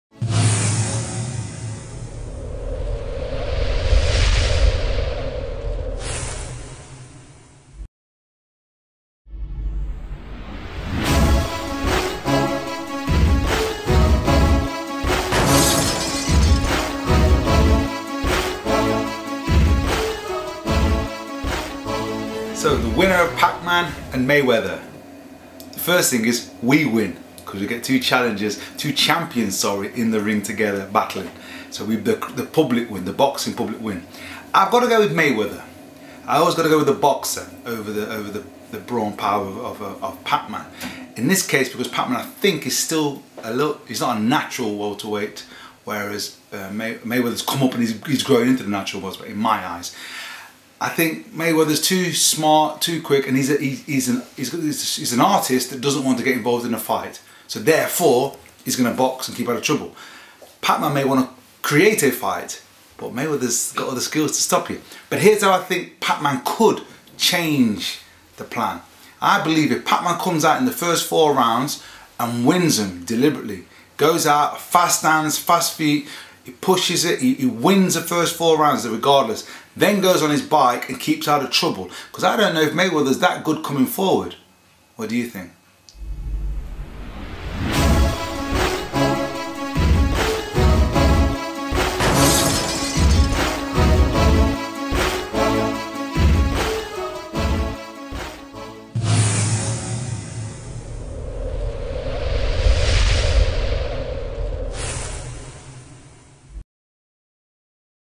Manny Pacquiao V Floyd Mayweather - Neville Brown
Former British Middleweight Champion Neville Brown Tells us Who Wins Manny Pacquiao V Floyd Mayweather